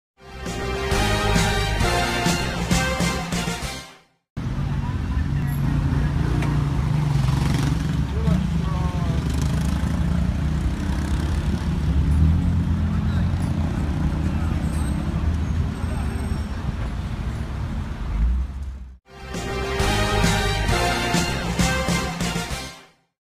Mujer atropellada en el centro
Fue hospitalizada